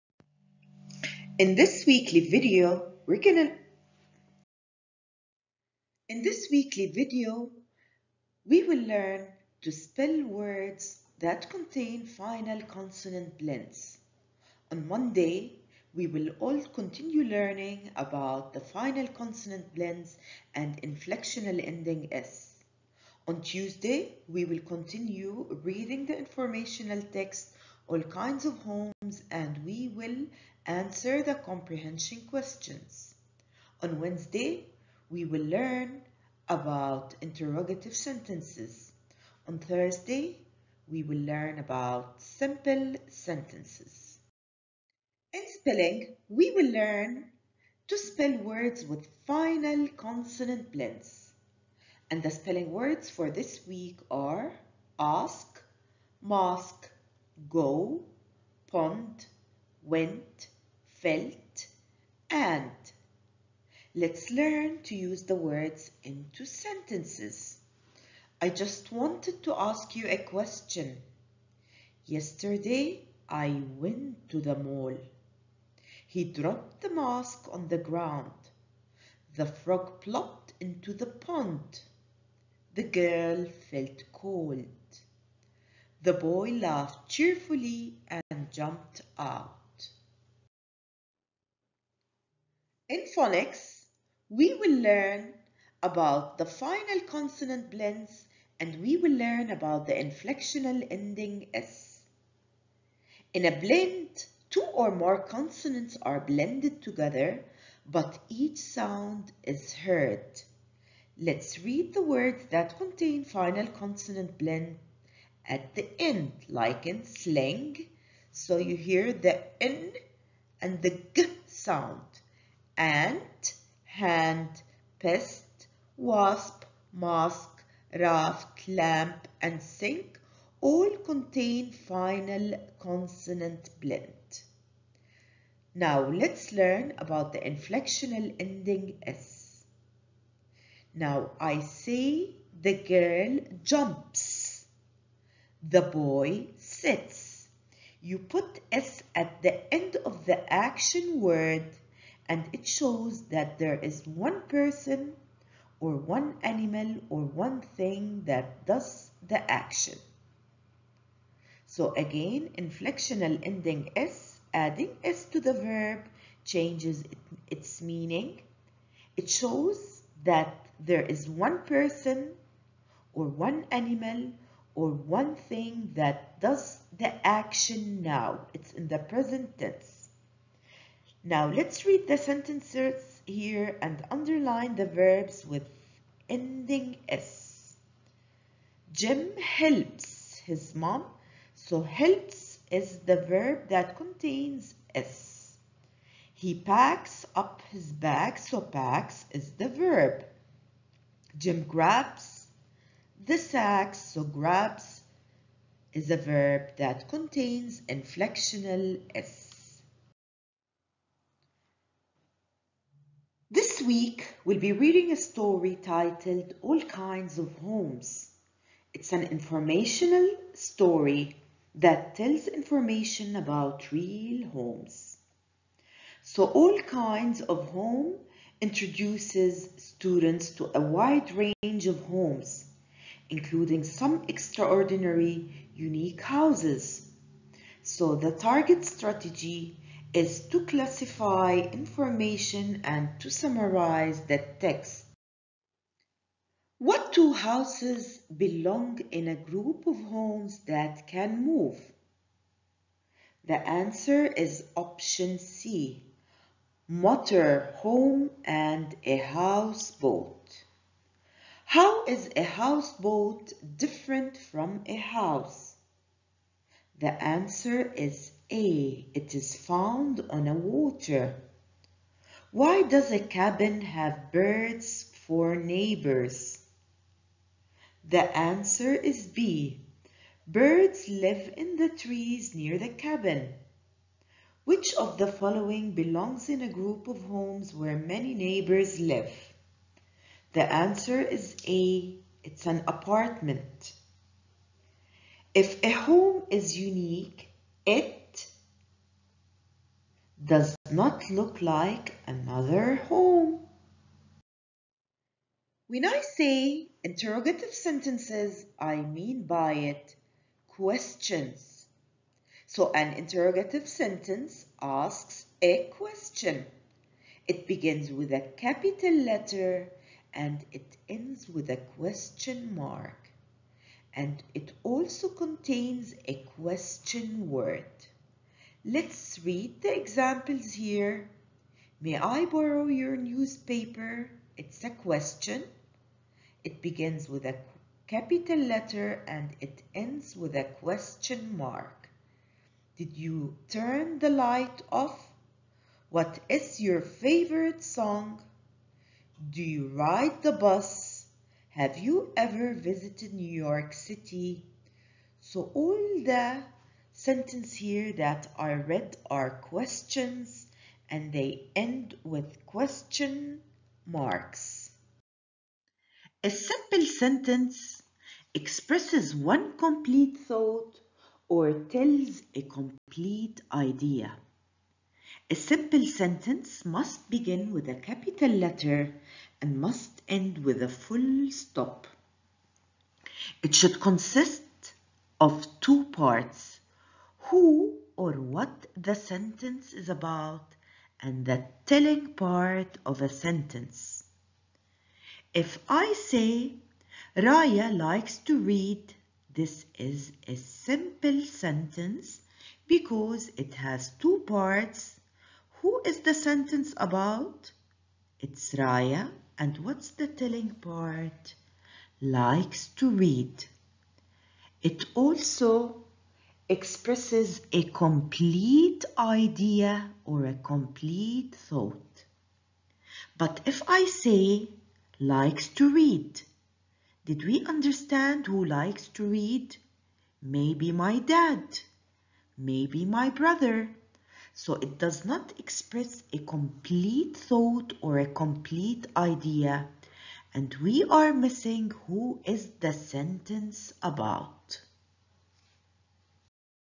English Weekly Video - Week 10 - Grade 1 - By Ms. Rima Al-Taher
English Weekly Video 10 (Nov.5th   -Nov.9th)

In this video, we'll delve into phonics, examining words with final consonant blends and action words that end with "s". We'll also read from "All Kinds of Homes" and discuss interrogative and simple sentences.